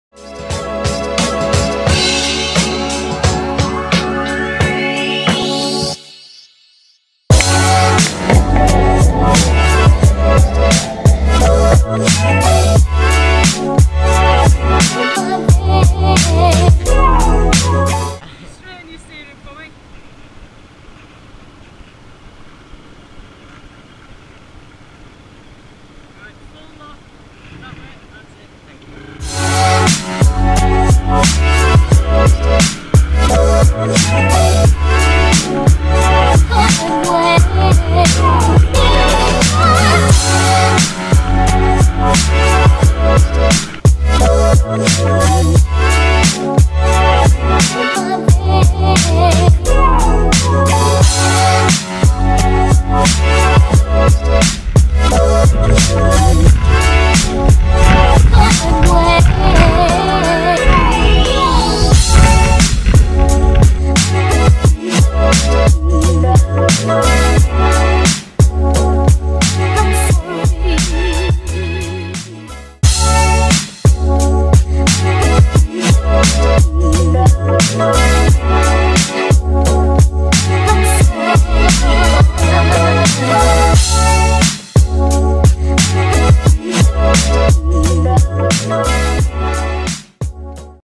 Centre parcs 2015 edit
Filmed our weekend at Centre Parcs Whinfell forest.